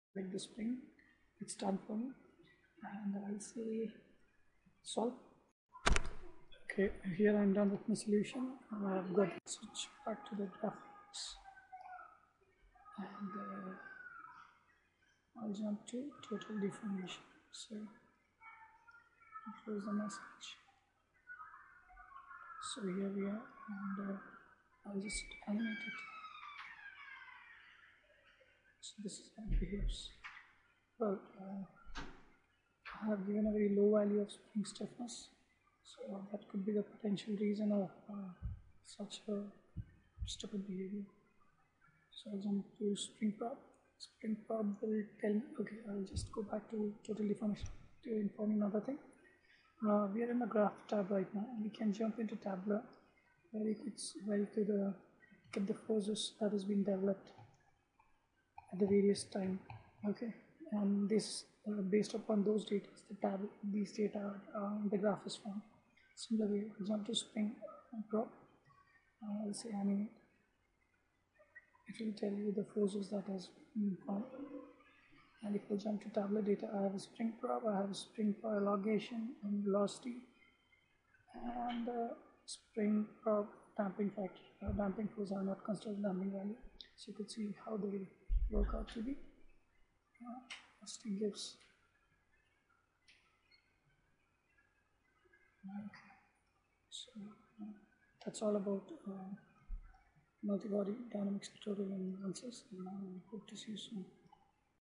Ansys Workbench Transient MUlti Body Dynamics-part 2
Ansys tutorial on transient MBD, Multi Body Dynamic explained using governor mechanism